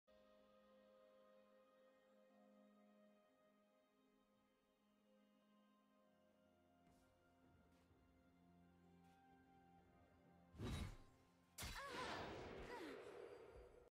My CANNON being secret agent working for blue team feat. Garen's LONG Sword.
Fake cannon cooperating with HUGE sword.